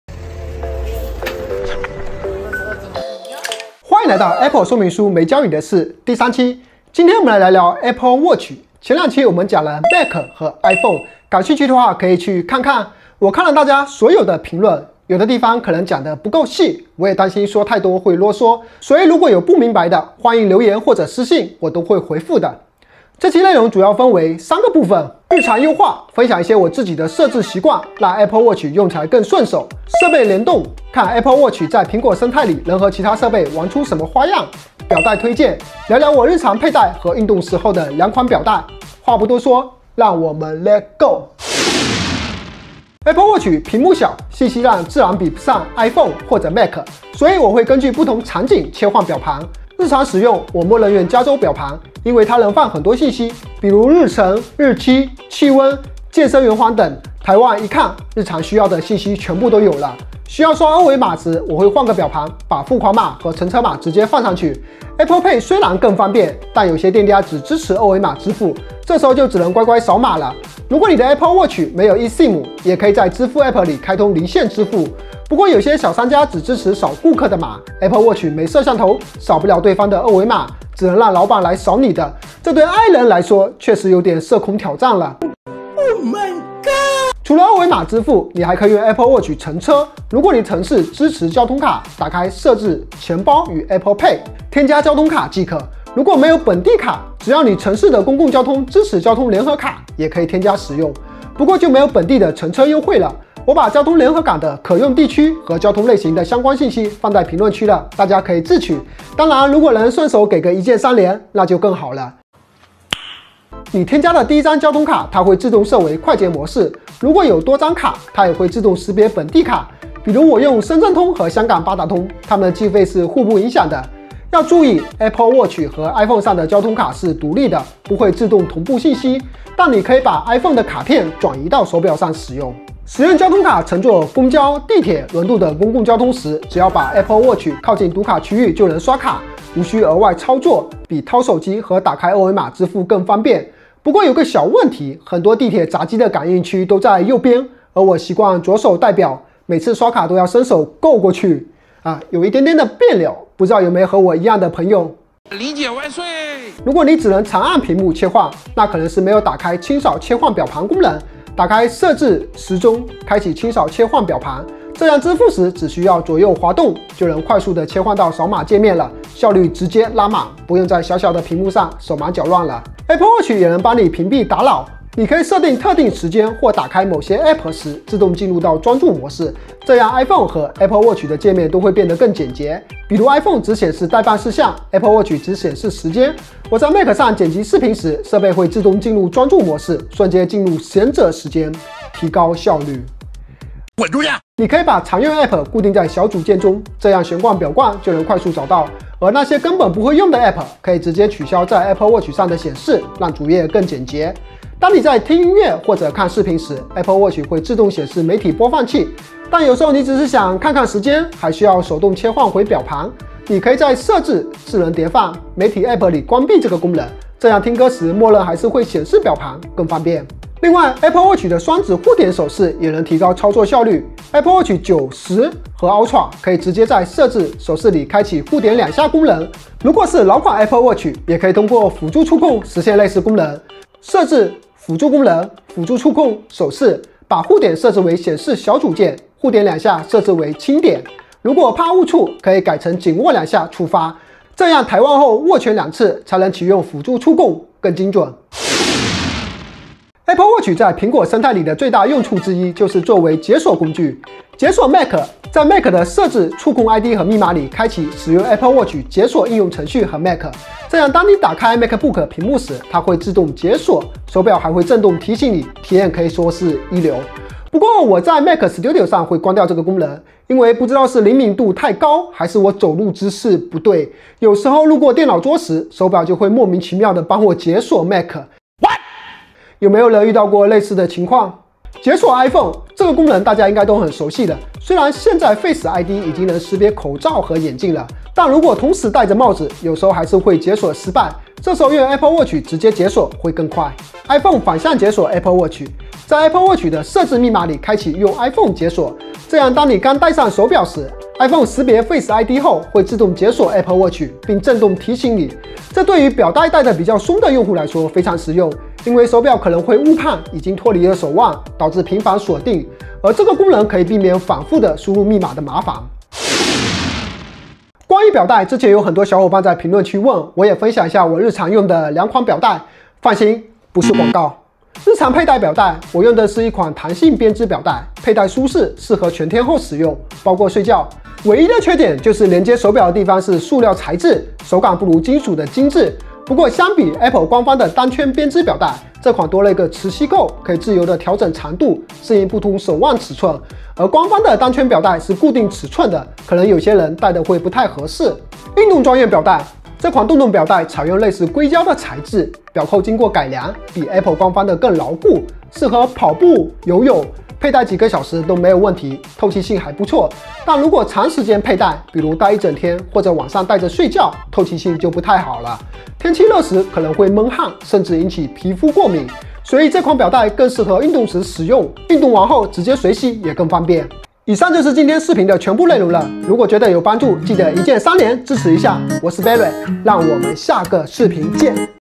隱藏在說明書之外的 Apple Watch 神設置！ | 最近使用
歡迎來到《Apple 說明書沒教你的事》第三期！ 我會從日常優化、蘋果設備聯動，帶你挖掘 Apple Watch 的隱藏玩法！ 最後，我還會推薦兩款高性價比錶帶，適合日常佩戴和運動場景。 話不多說，快來看看你的 Apple Watch 還能多強大吧！
0:00 觀看指南
00:41 日常優化
04:08 設備聯動
05:16 錶帶推薦